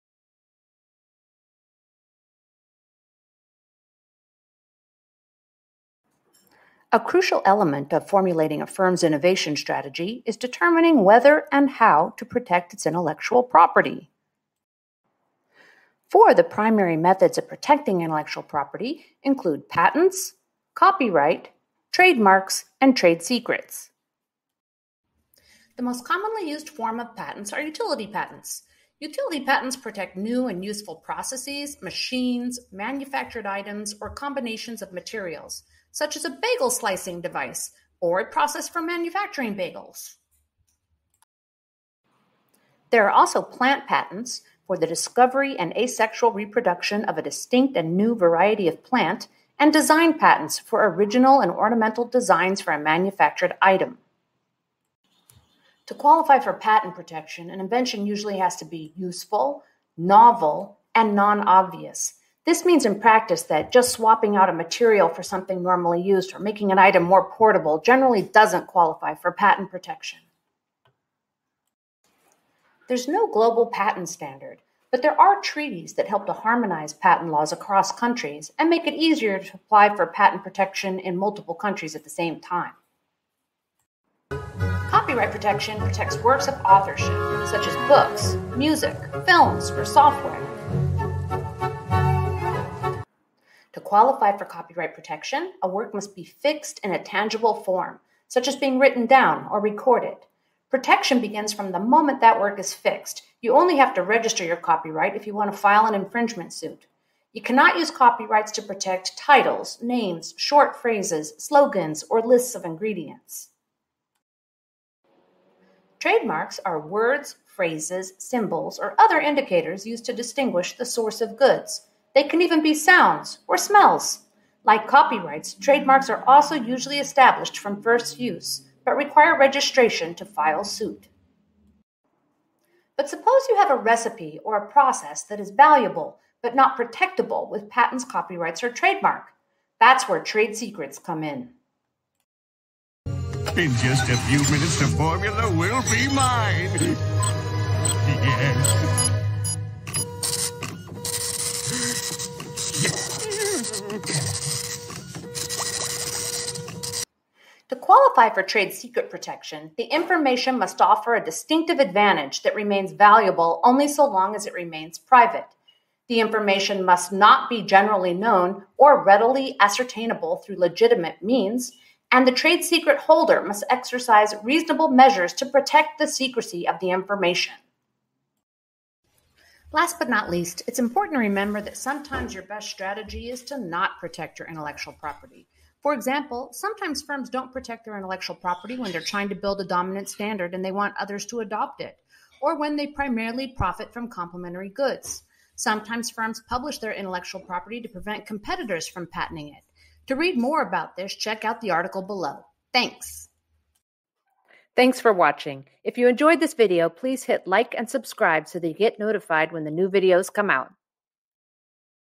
Innovation Strategy: Intellectual Property Protection
This short animated video explains the major intellectual property protection methods available, when they are used, and qualifications for using them. The creator is the author of the textbook, Strategic Management of Technological Innovation.

Note, this version is a slightly cleaned up version of the original.